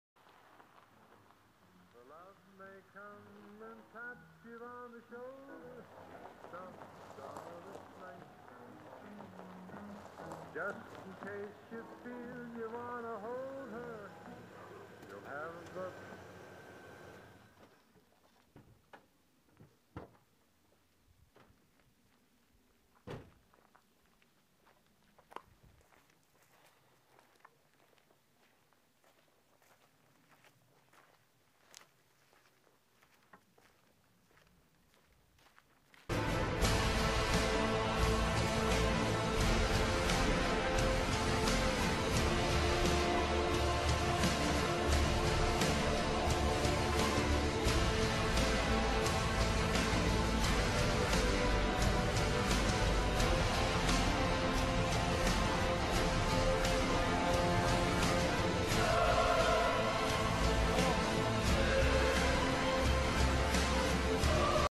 Advanced Digital Crane Jib - 8/12 ft Jib (TEST)
Here are some tests using the Canon T3i on the Advanced Digital 8 to 12 foot Jib! For $240 this Jib is amazing! It does everything you would expect from a 12 foot jib and is very easy to use/set up!